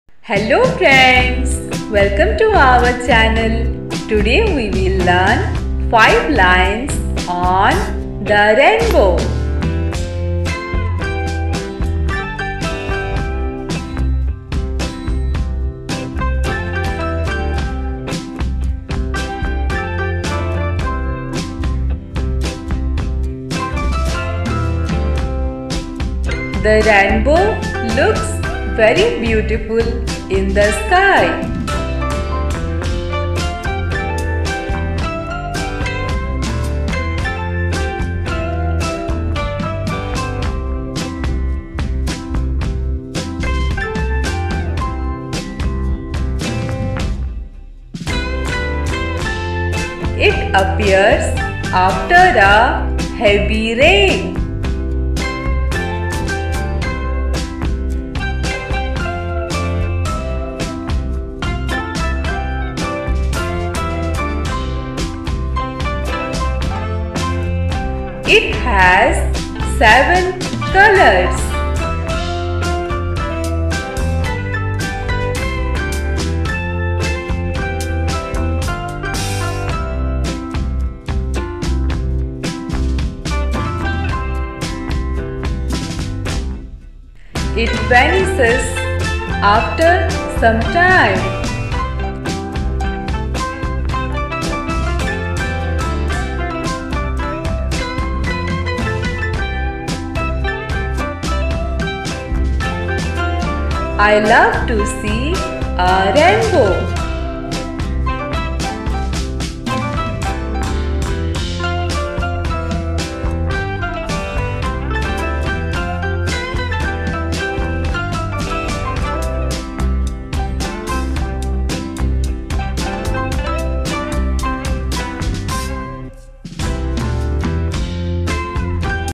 5 line indradhanush par english mein | Essay on rainbow 5 lines
Hello friends! This video is about 5 lines on the rainbow in English or "5 line indradhanush par english mein". This is a very easy essay writing. You will learn it very nicely.